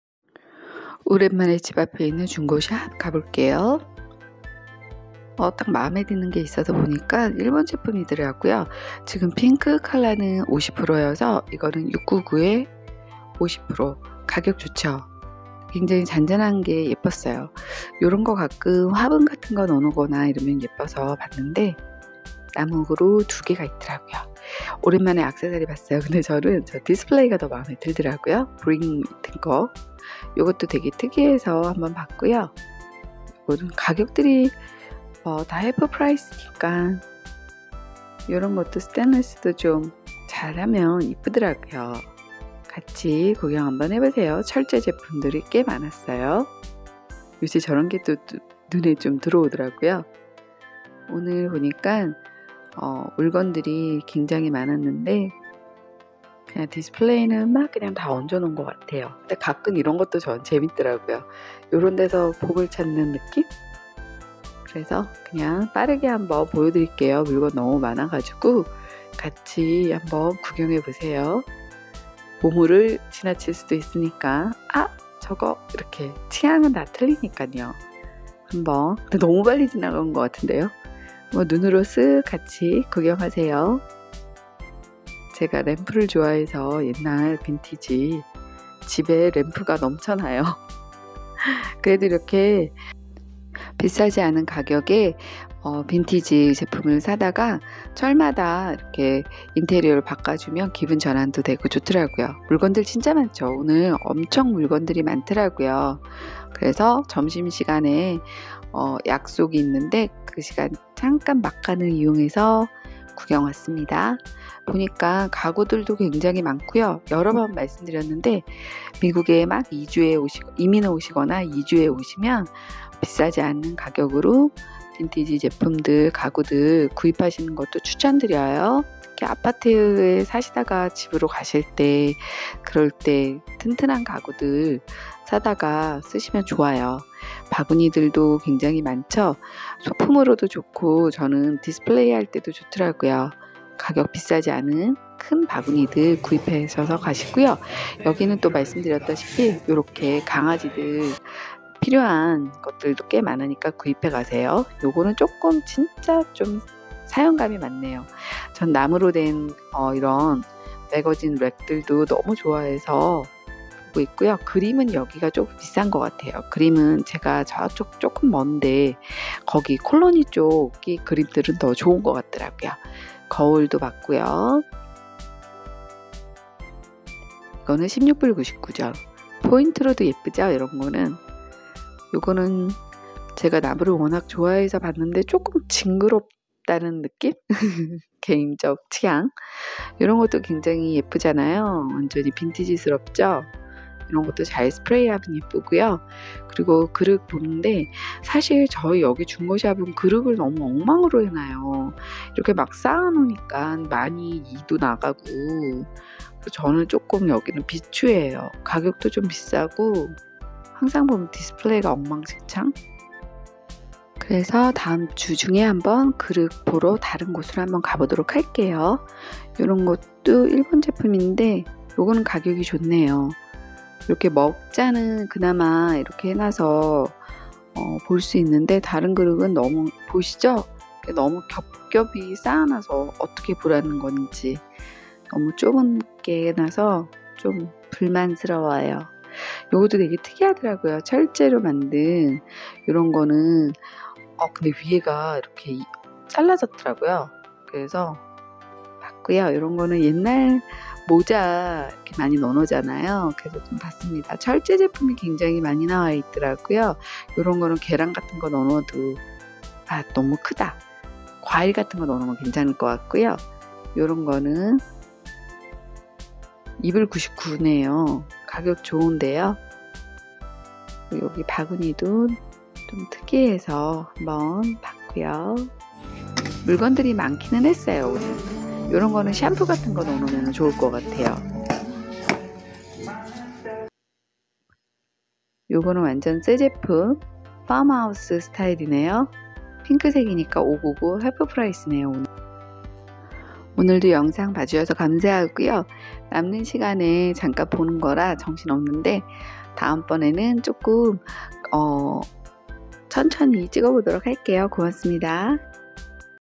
오늘은 어떤 물건들이 있을까?같이 구경해 보세요: 미국 달라스 중고샵
점심 약속 30분전 급하게 동네 중고샵 한번
아이쇼핑 갑니다~

딱히 살 거 없지만 구경만으로도 재미나요

오늘도 영상 봐주셔서 고맙습니다!